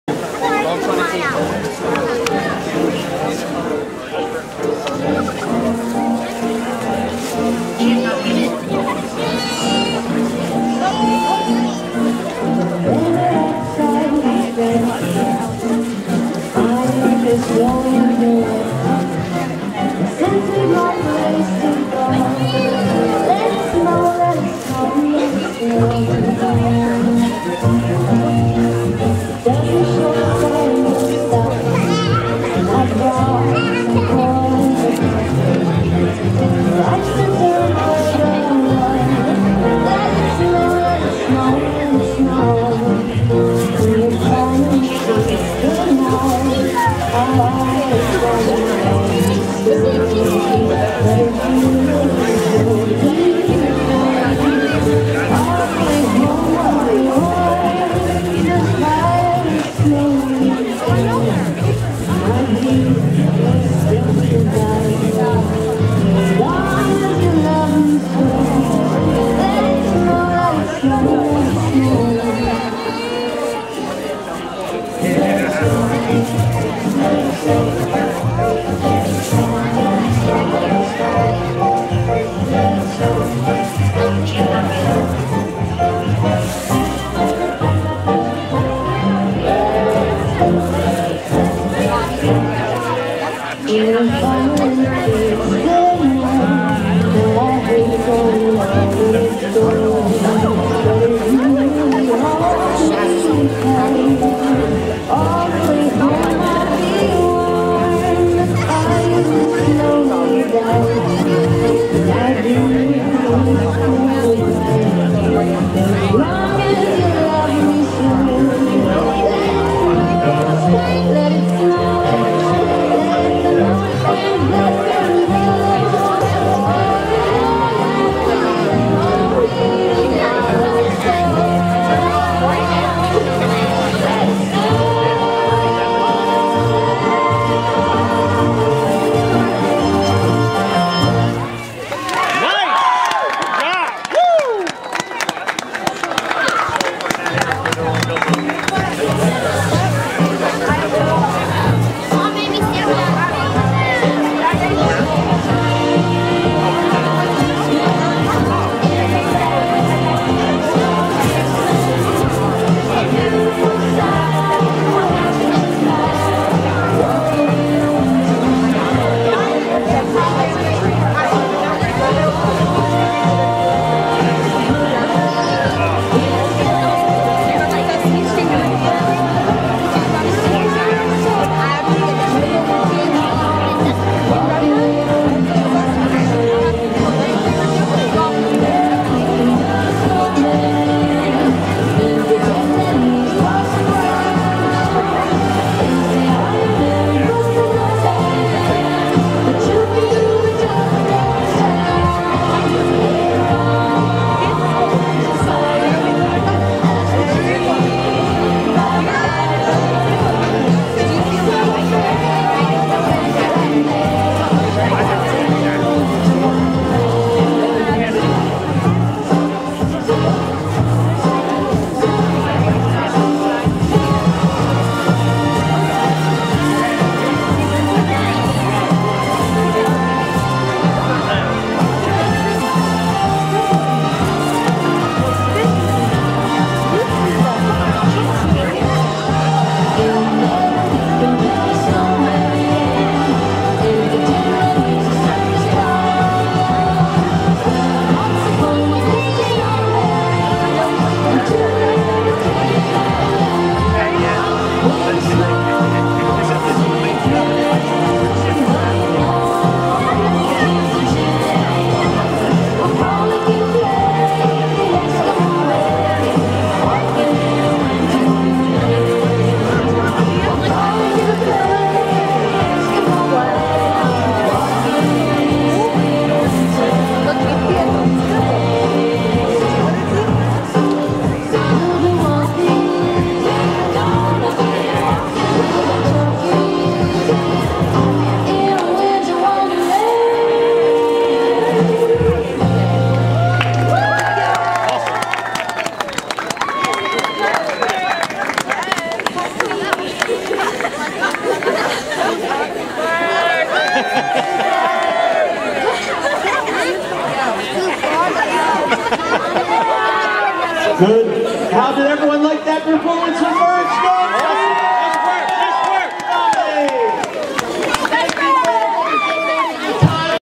Merge Dance Studio's Performance at Roxborough United Soccer Club's Inaugural Tree Lighting Ceremony
Merge Dance Studio's performance group, Merge Dance Company, performed at the Roxborough United Soccer Club's Inaugural Tree Lighting Ceremony at Houston Playground in Andorra on 12/9/18.